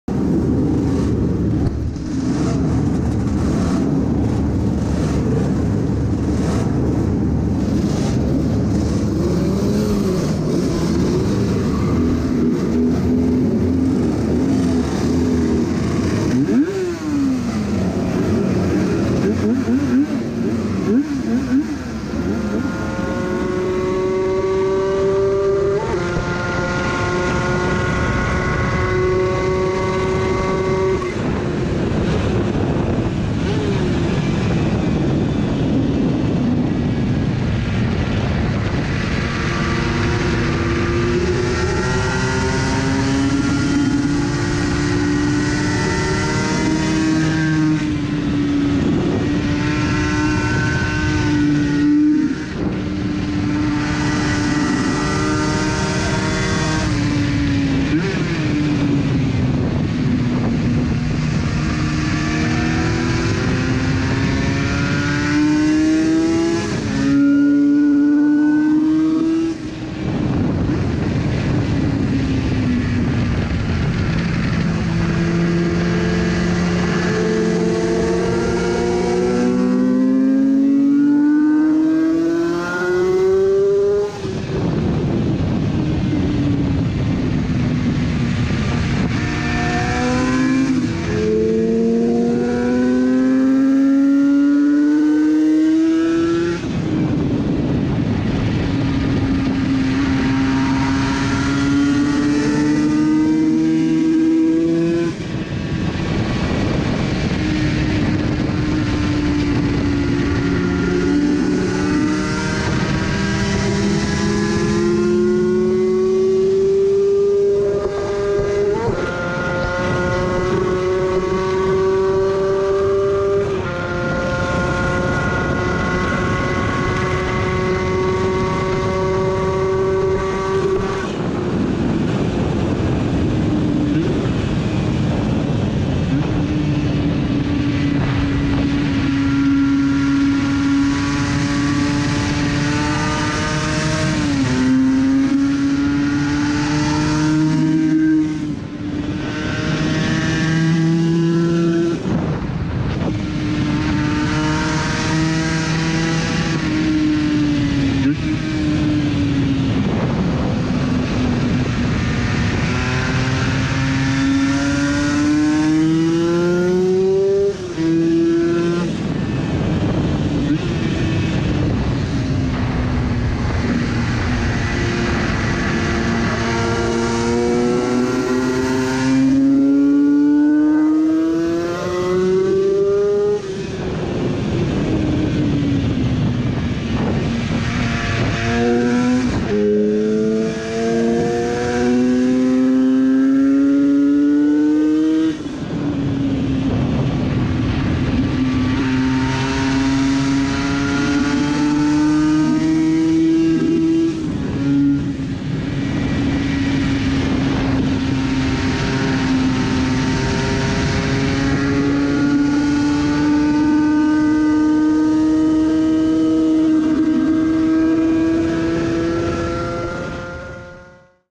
Ride along Johnny Cecotto 500cc. in Dijon
Ride along Johnny Cecotto with the Yamaha 500cc during the Moto Legende Revival in Dijon, France the 3 June 2018.